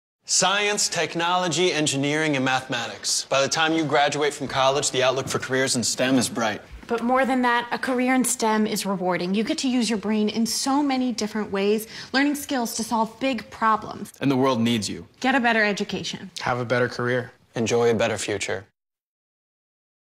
The Flash Cast Interview Grant Gustin, Carlos Valdes and Danielle Panabaker
Science Fiction